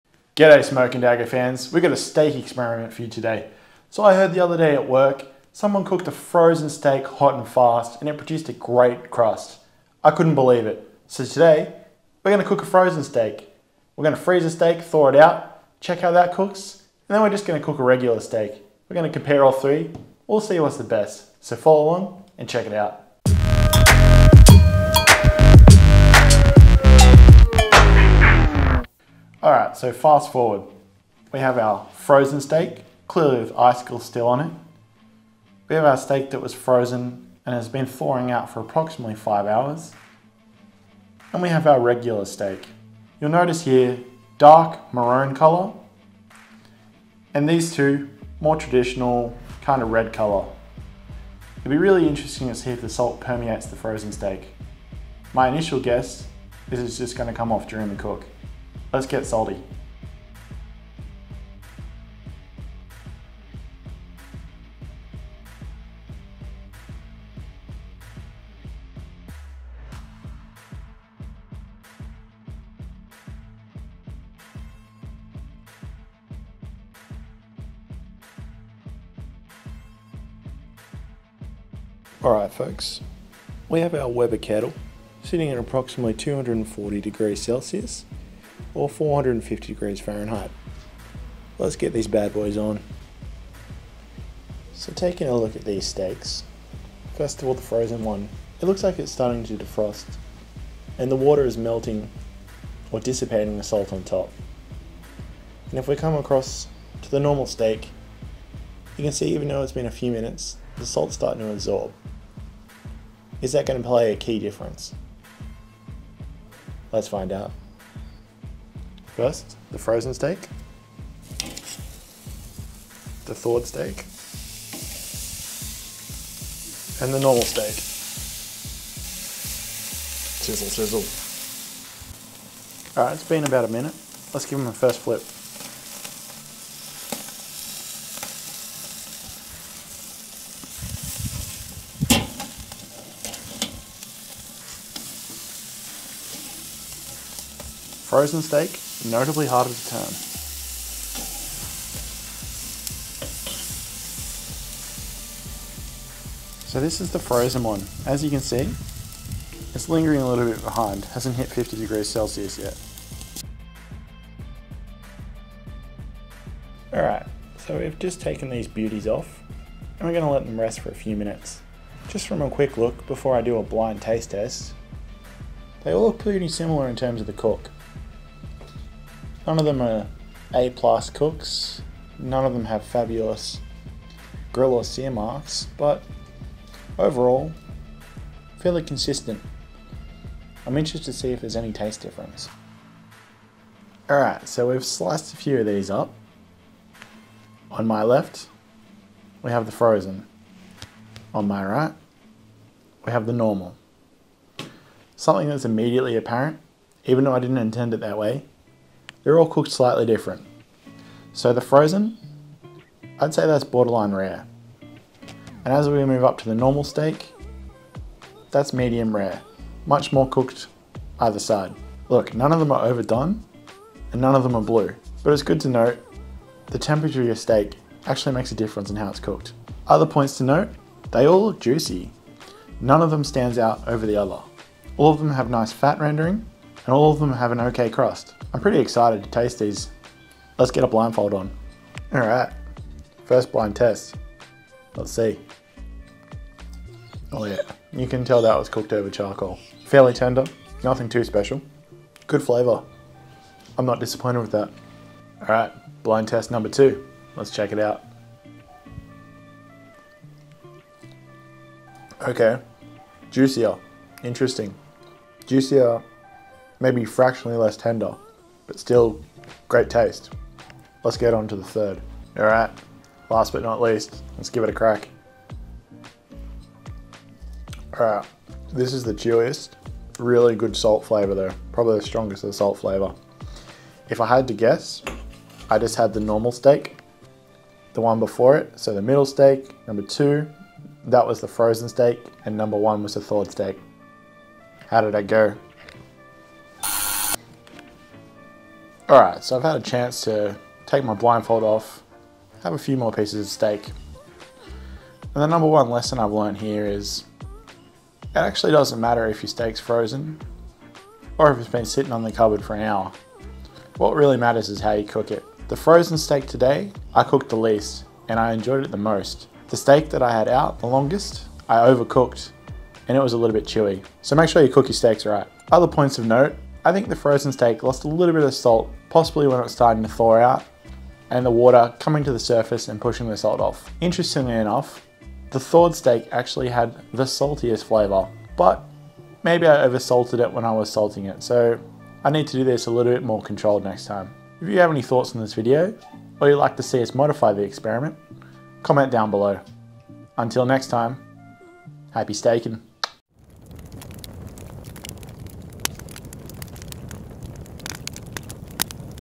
Does freezing your steak before grilling make it better?
Join the lads at Smoke and Dagger as they get their science on, testing if a frozen steak is as good, or better than a room temp steak for grilling.